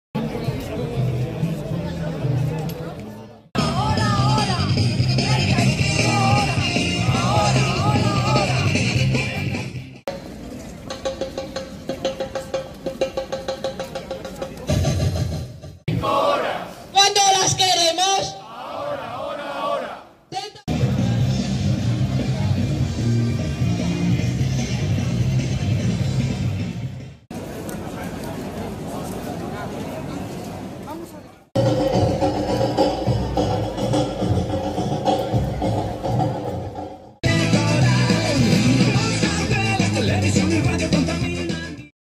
Unos 150 funcionarios piden en Valladolid la recuperación de derechos
Los empleados públicos reclaman en una manifestación  la recuperación de derechos laborales